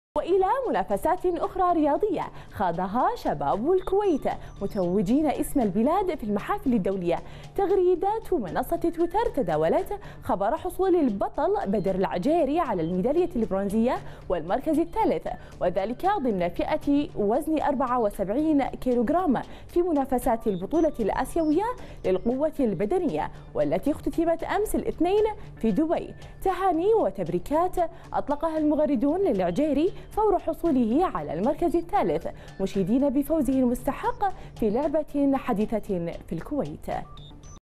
بدر_العجيري يقتنص برونزية البطولة الآسيوية في دبي
تلفزيون الكويت: بدر_العجيري يقتنص برونزية البطولة الآسيوية في دبي